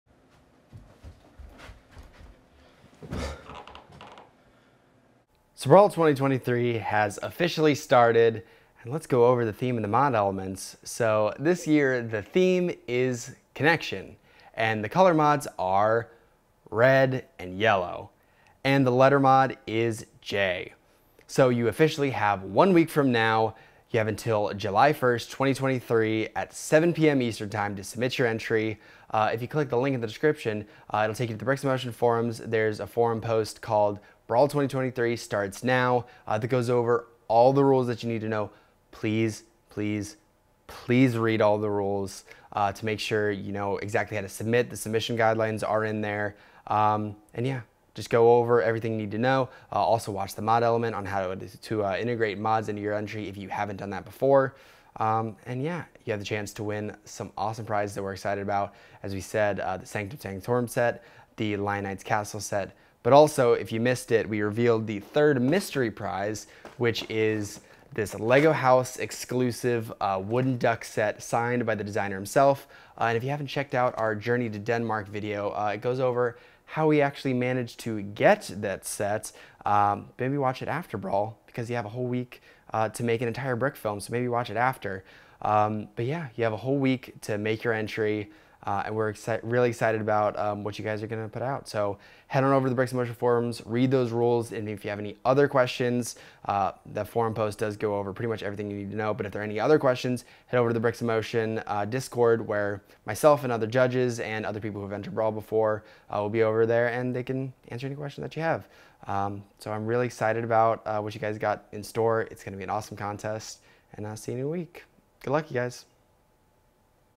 BRAWL 2023 - Theme & Mod Announcement (BRAWL IS OVER!)
BRAWL 2023 HAS STARTED! Check out the theme/mods, and get to work!